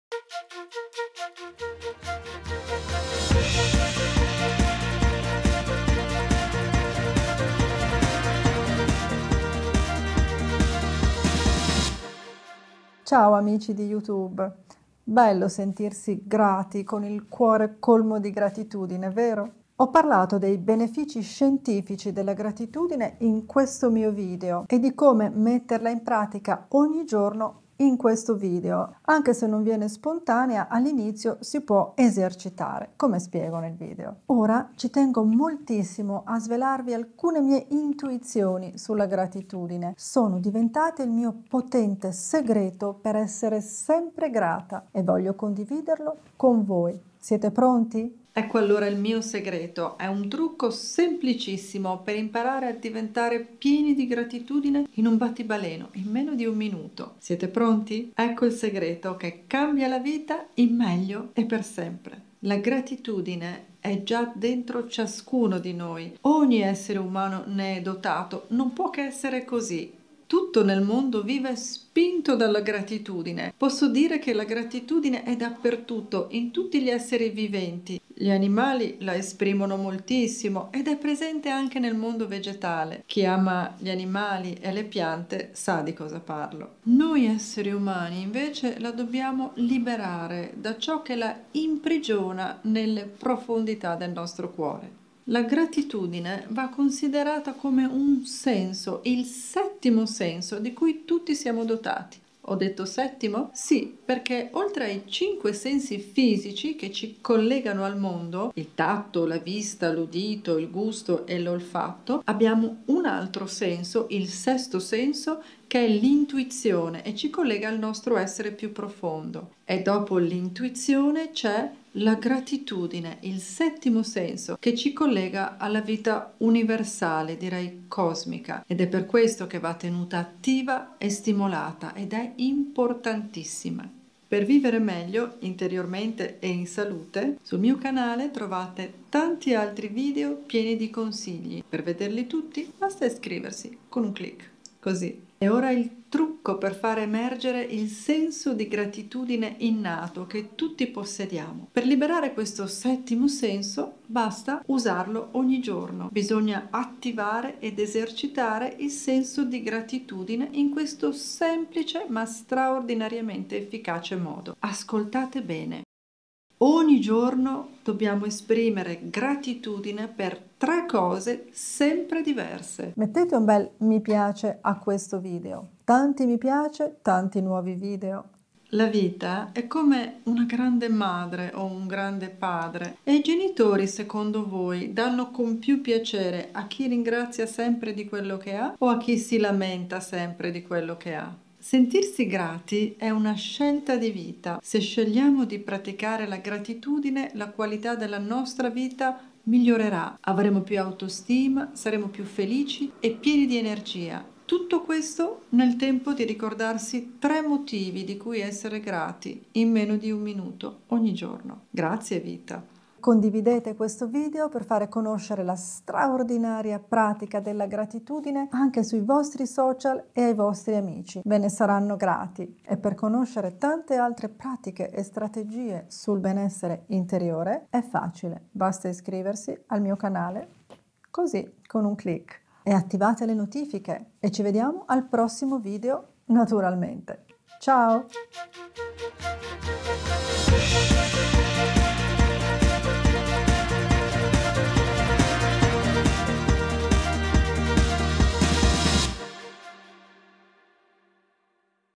VLOG GRATITUDINE PARTE 3: 1 SEGRETO SPECIALE per VIVERE la GRATITUDINE ogni giorno By Simona Vignali
La gratitudine è uno degli strumenti più efficaci e piacevoli per vivere una vita di soddisfazione, autostima, energia, positività e felicità. Essere grati alla vita per quello che abbiamo, poco o tanto che sia, è un insegnamento prezioso che oggi sembra difficile ricordare.

Scopri in questo video 1 speciale esercizio per coltivare la gratitudine ogni giorno.

Nota bene

Simona Vignali